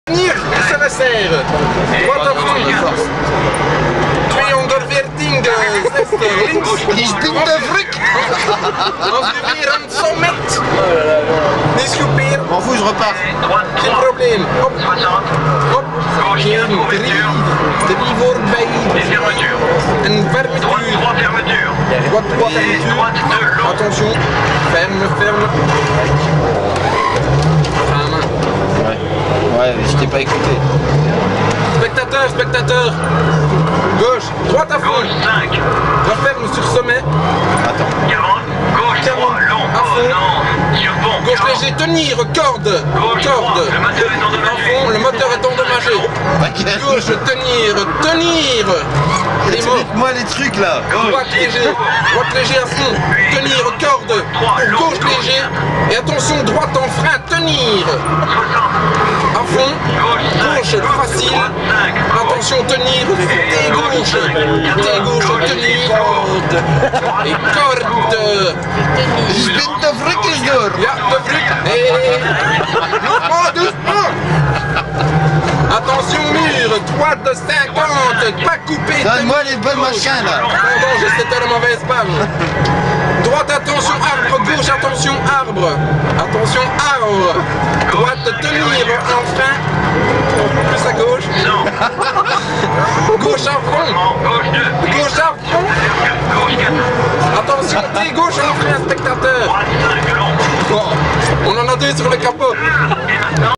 Motorshow de Liège ES2
Playmobil se charge de la conduite alors que Joups est toujours au notes